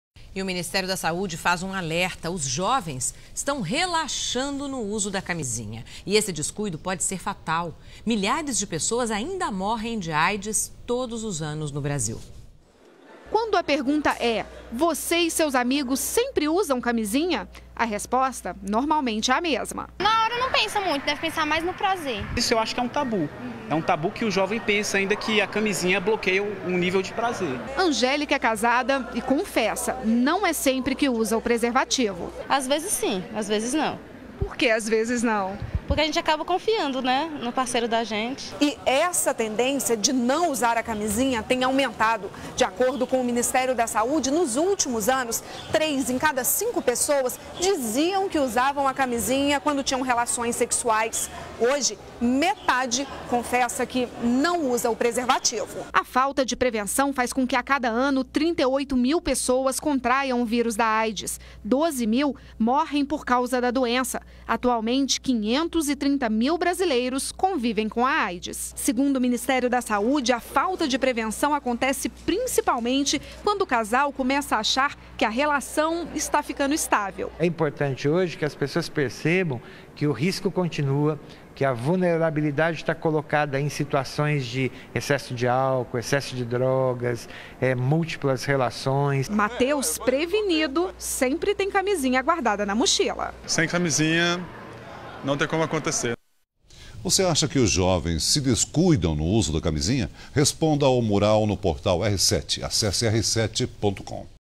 Ministério da Saúde diz que jovens estão relaxando no uso da camisinha
O ministério da Saúde faz um alerta: os jovens estão relaxando no uso da camisinha e o descuido pode ser fatal. Milhares de pessoas ainda morrem de Aids todos os anos no Brasil.